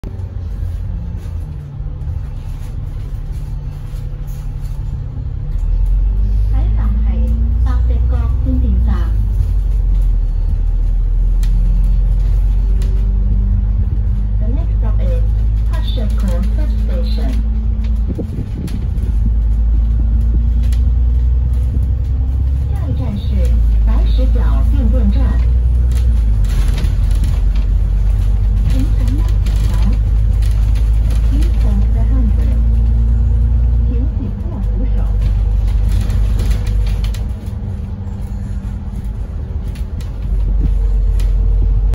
【只限平日繁忙時間服務，屬263線之特別班次】九巴263C線 - 白石角變電站（S1） + 請緊握扶手 報站（UD4786）
（粵）下一站係白石角變電站。
（英）The next stop is Pak Shek Kok Substation.
（普）下一站是白石角变电站。